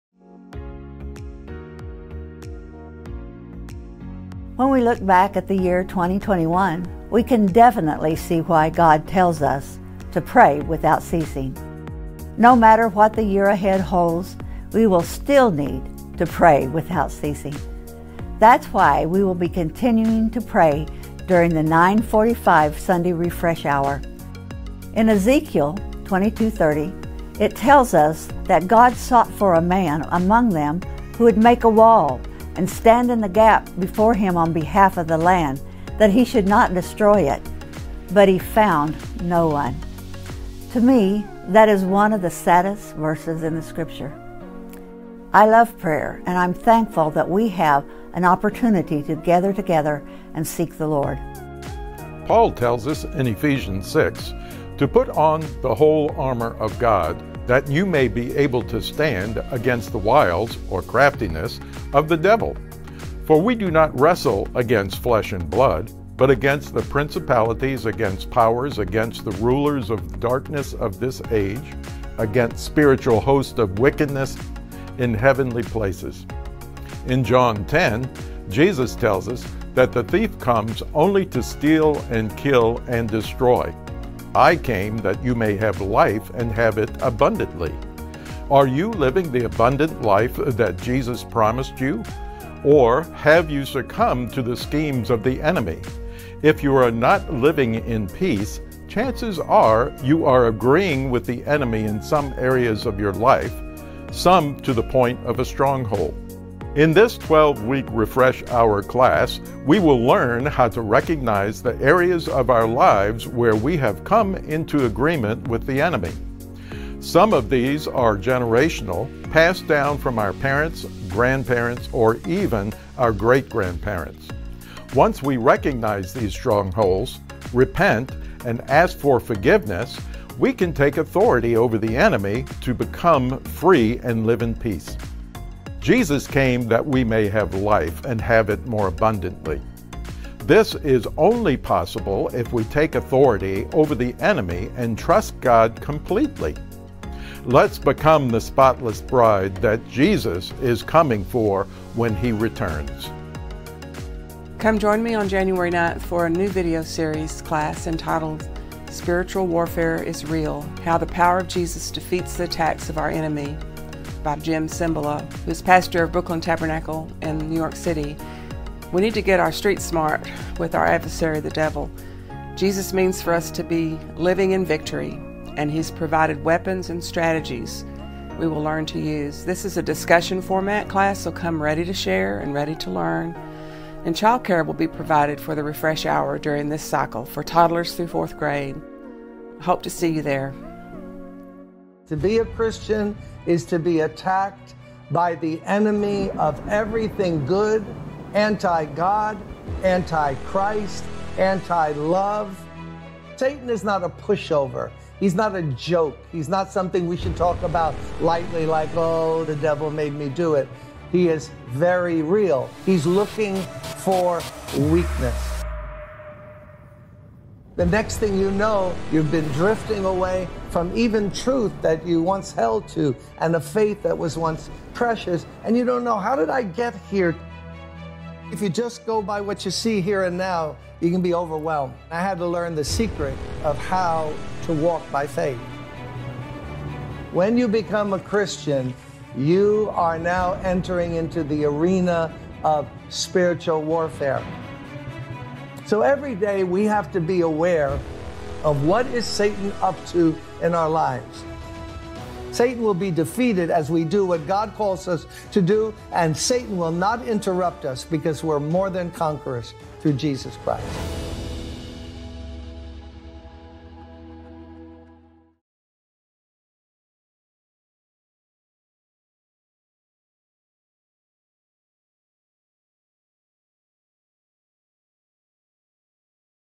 CCF Winter Refresh Hour Classes Begin Jan. 9, 2022
Three new opportunities are available during CCF's Sunday morning Refresh Hour in this first quarter of 2022: 1) Prayer for the Nation, led by Nancy Nash, 2) Breaking Strongholds by Don Clark, and 3) "Spiritual Warfare is Real" video series by Jim Cymbala, facilitated by LuAnn Hickey.

Community Christian Fellowship Service